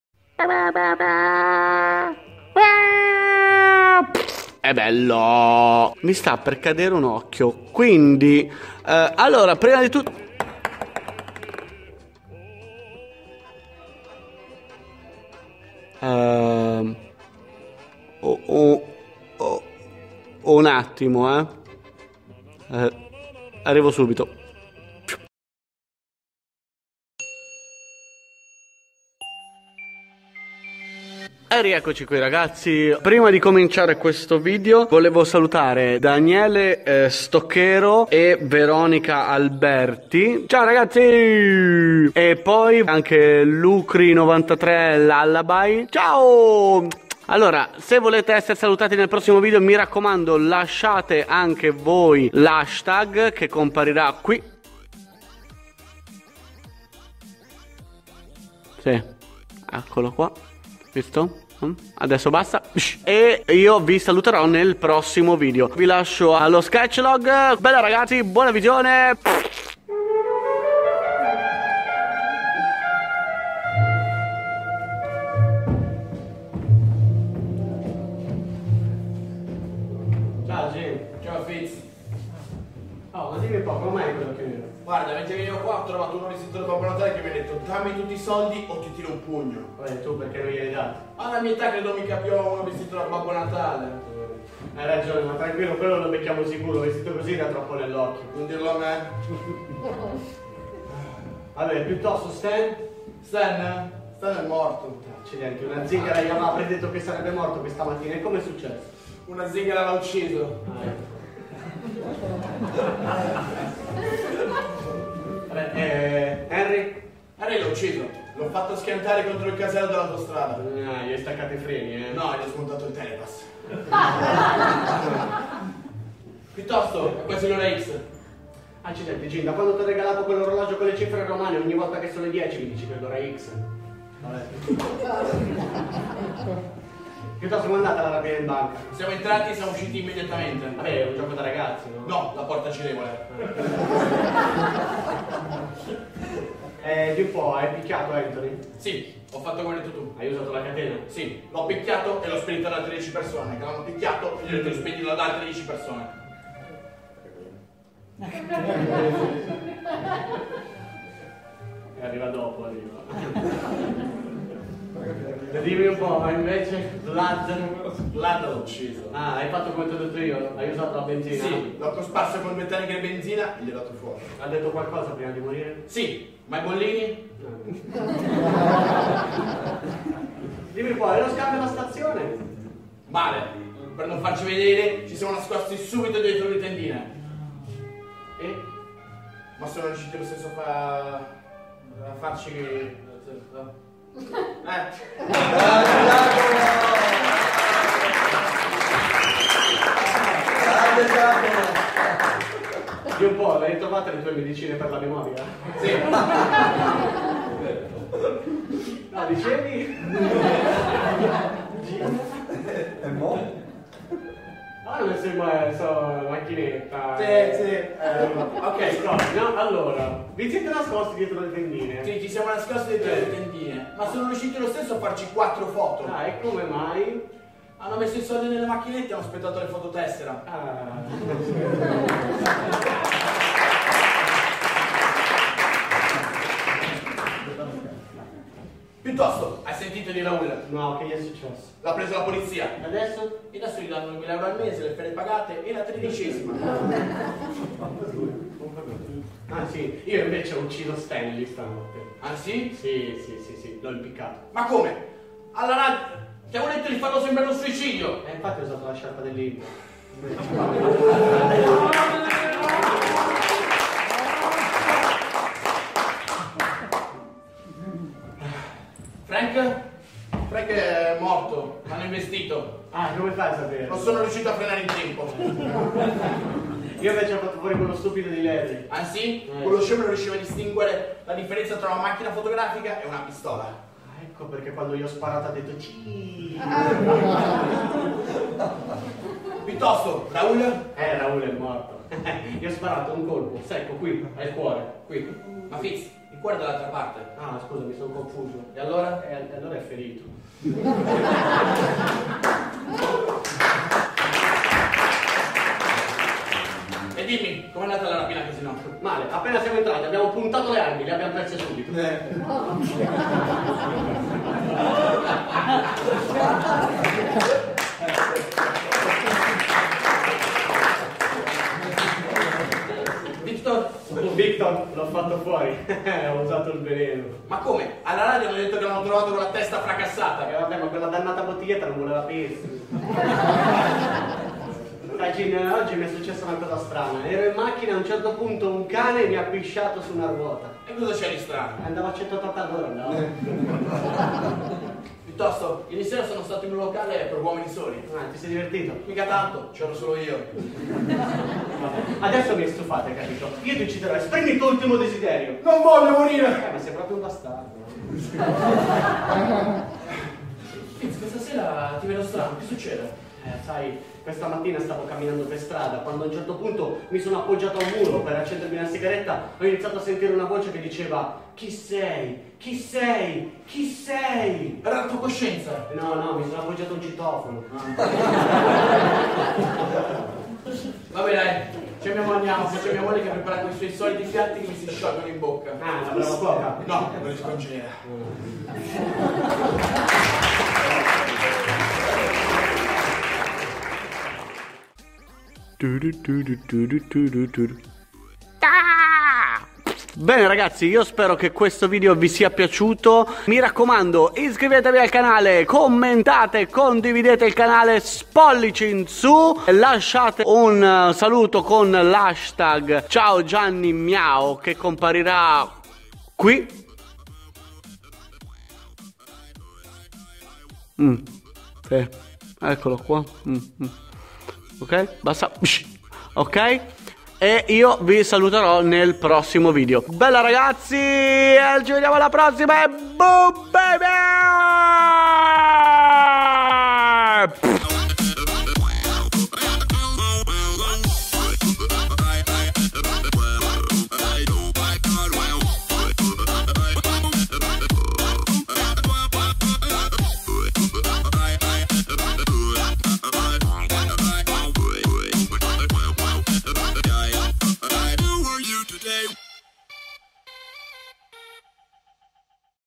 SKETCHLOG #7 - A TEATRO CON I GANGSTER! w/GIACO
Bella a tutti ragatti! Ma chi e' arrivato in teatro stavolta?!?! Lasciate un commento con l' hastag CiaoGianniMiao e vi saluteroò nel prossimo video!
!! BOOM BABY!!